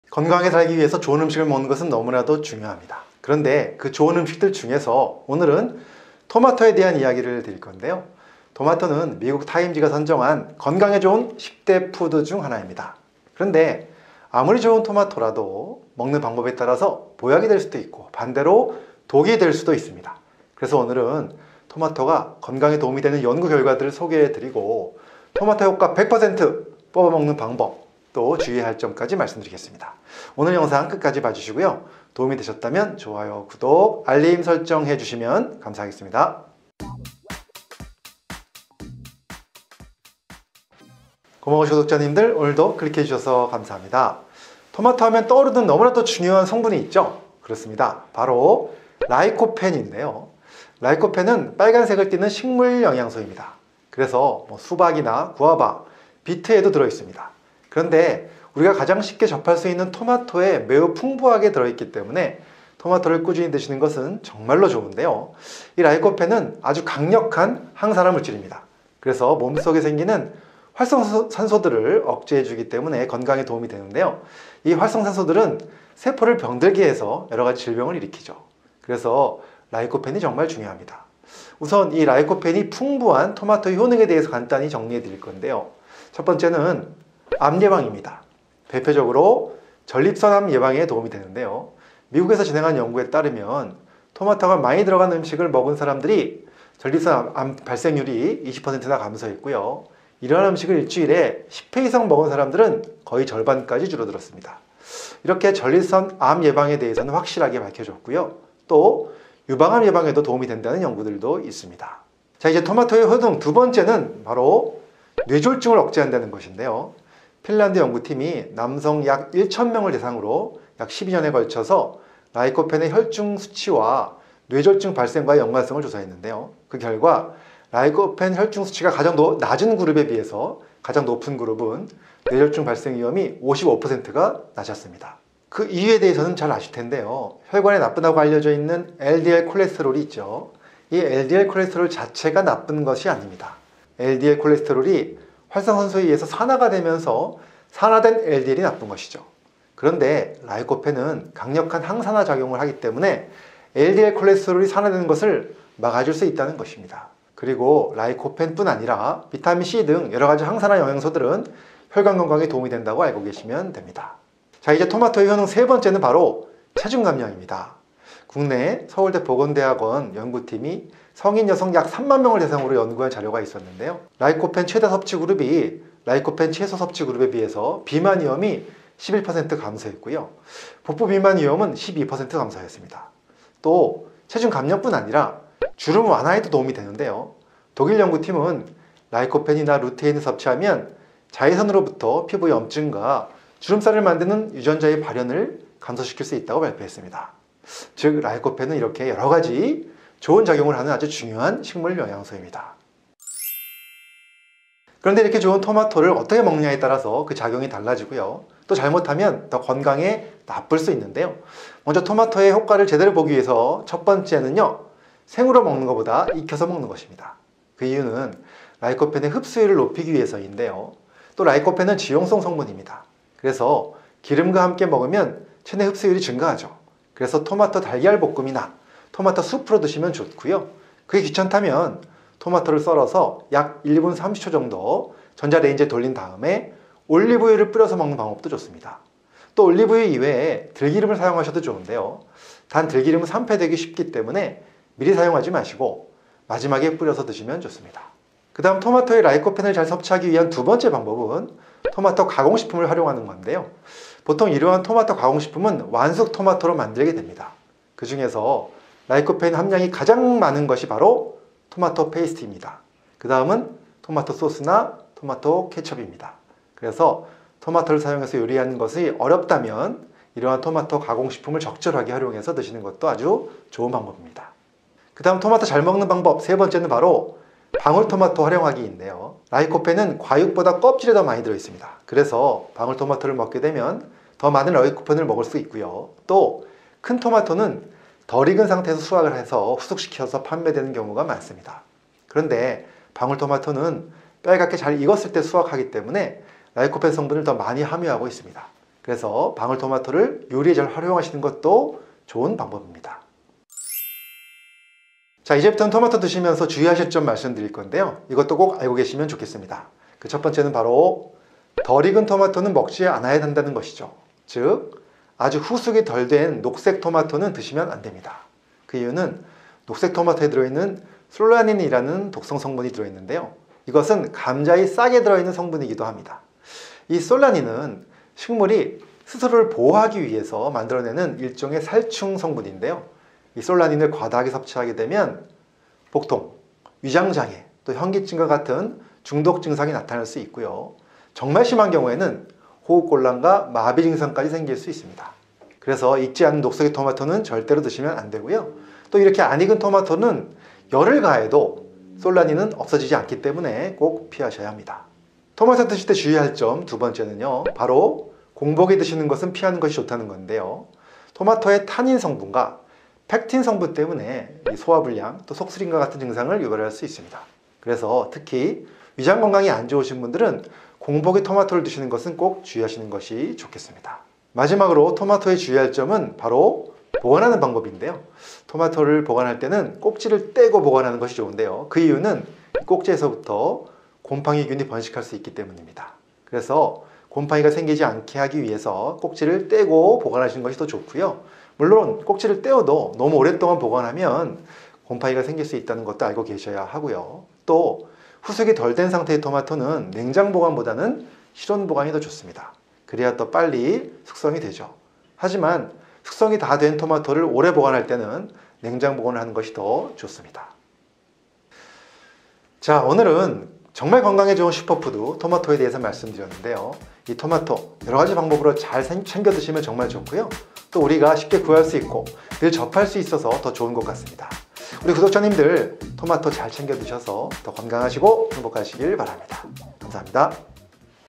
토마토 효과 100% 뽑아먹는 방법. 항암 항산화 뇌졸중 예방 효과가 확 높아집니다.
"토마토, 진작 이렇게 먹었더라면" 토마토와 '이것' 함께 먹으면 온갖 염증 사라집니다. 토마토 효과 100% 뽑아먹는 방법. 항암 항산화 뇌졸중 예방 효과가 확 높아집니다.
00:00 오늘의 주제 소개
01:26 토마토의 첫 번째 효능
01:52 토마토의 두 번째 효능
02:44 토마토의 세 번째 효능
03:31 토마토 효능 제대로 보는 첫 번째 방법
04:08 토마토 효능 제대로 보는 두 번째 방법
04:34 토마토 효능 제대로 보는 세 번째 방법
05:09 토마토 섭취 시 주의할 점1
05:55 토마토 섭취 시 주의할 점2
06:14 토마토 섭취 시 주의할 점3